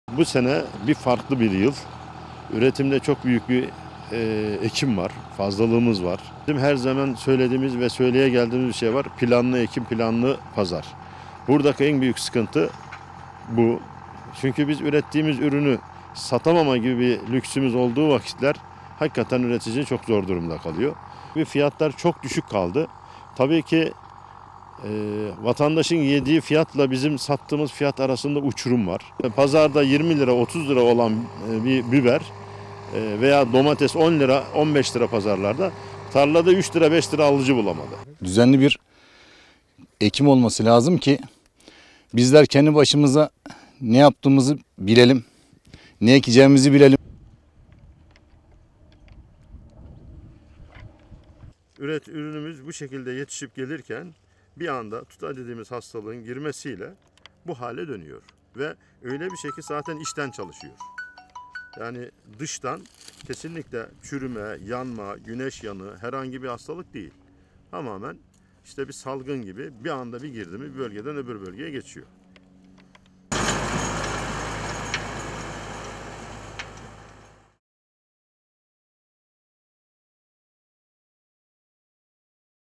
Tarlada 3, Pazarda 30 Lira, Alıcı Bulamayan Çiftçi Ürününü Sürdü!
Eskişehir'de bazı çiftçiler plansız üretim ve maliyetlerden dolayı ürünlerini hasat etmeden tarlalarını sürerken, pazarda fiyatı 15 lira ile 30 lira arasında değişen sebzeyi 3 liraya satamadıklarını belirtti.

Eskişehir'de pazarda biber domatesinin kilogramı 15 ile 30 lira arasında değişirken, çiftçi 3 liraya alıcı bulamadığını dile getirdi. Hasat yapmayan bazı üreticiler, tarlada çürüyen sebzelerinin bir kısmını traktörle sürdü.
Vatandaşın satın aldığı ile tarladan satılan ürün fiyatları arasında fiyat farkının ciddi düzeyde olduğuna değinen Yeşil Sakarya Üreticiler Birliği Başkanı Süleyman Buluşan, "Bu sene farklı bir yıl oldu. Üretimde çok büyük bir ekim fazlalığımız var. Her zaman söylediğimiz bir şey var; o da planlı ekim, planlı pazardır. Buradaki en büyük sıkıntı budur. Ürettiğimiz ürünü satamama gibi bir lüksümüz olduğu zaman üretici çok zor durumda kalıyor. Fiyatlar çok düşük kaldı. Vatandaşın yediği fiyatla bizim sattığımız fiyat arasında bir uçurum var. Pazarda 20 ila 30 lira olan biber veya 10 ila 15 lira olan domates, tarlada 3 liraya alıcı bulamadı." dedi.
Tarlasındaki biberi hasat etmeden traktörle süren çiftçi Faruk Doğruparmak da plansız üretimden dert yandı. Doğruparmak, "Düzenli bir ekim olması lazım ki bizler ne yaptığımızı bilelim, ne ekeceğimizi bilelim" dedi.